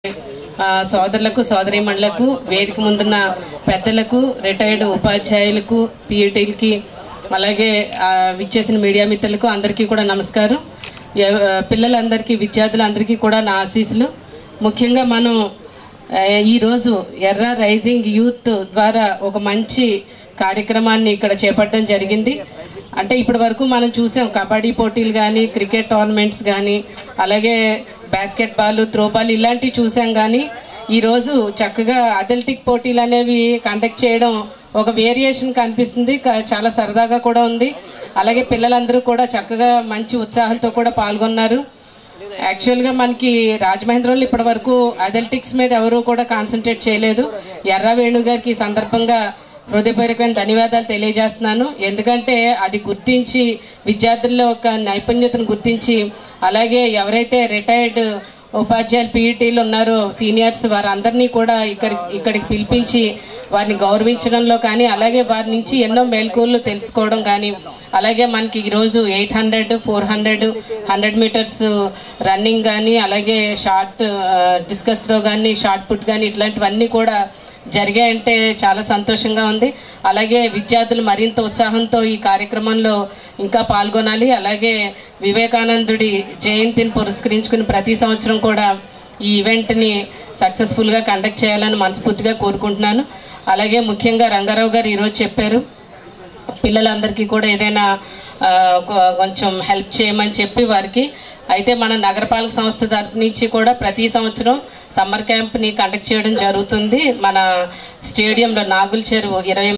ATHELETIC MEET AT RAJAHMUNDRY
ATHELETIC  MEET AT RAJAHMUNDRY  by yerra's  rising youth held on 29/1/2018